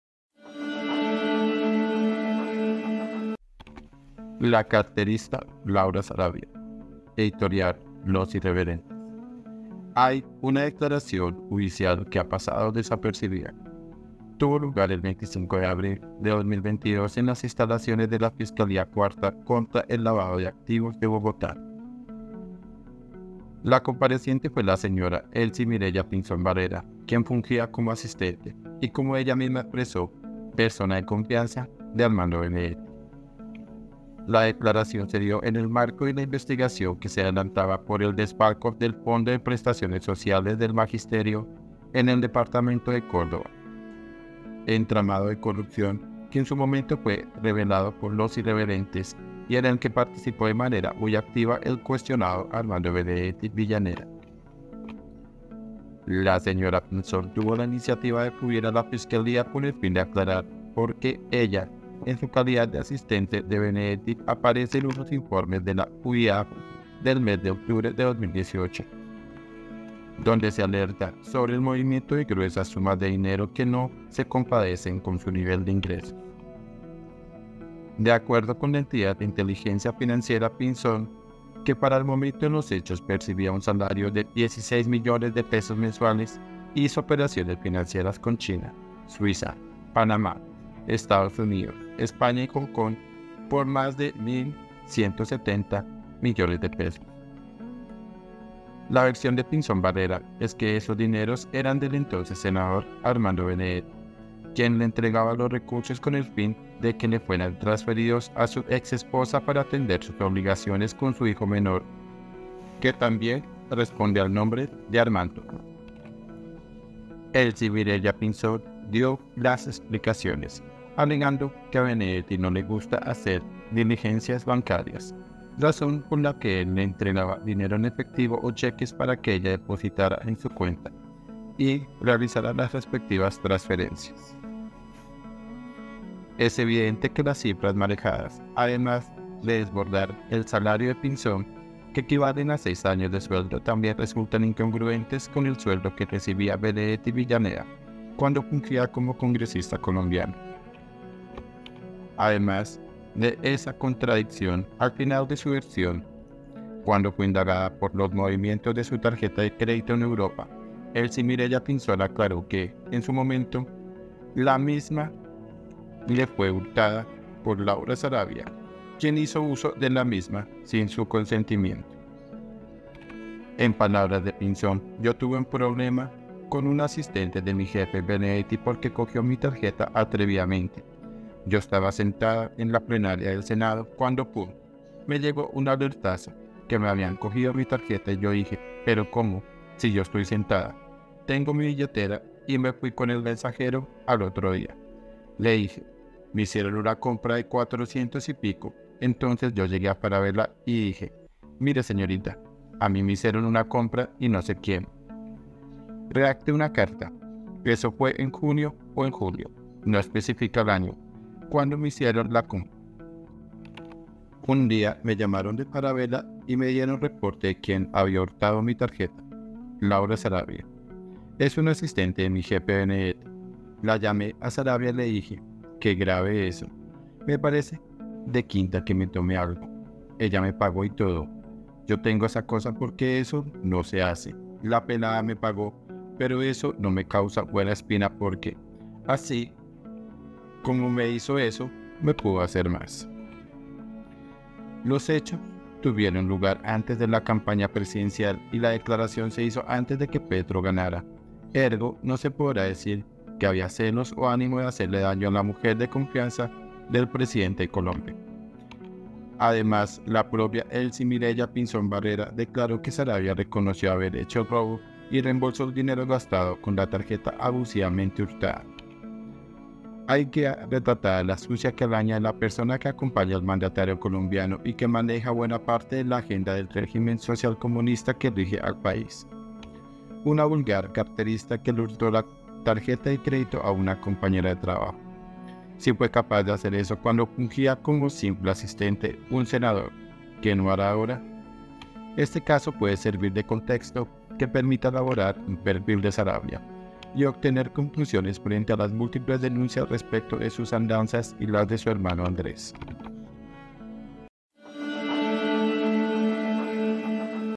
LA CARTERISTA LAURA SARABIA ⛔ Editorial Los Irreverentes
Ahí queda retratada la sucia calaña de la persona que acompaña al mandatario colombiano y que maneja buena parte de la agenda del régimen socialcomunista que rige al país. Una vulgar carterista que le hurtó la tarjeta de crédito a una compañera de trabajo. ¿Si fue capaz de hacer eso cuando fungía como simple asistente un senador, qué no hará ahora? Este caso puede servir de contexto que permita elaborar un perfil de Sarabia, y obtener conclusiones frente a las múltiples denuncias respecto de sus andanzas y las de su hermano Andrés.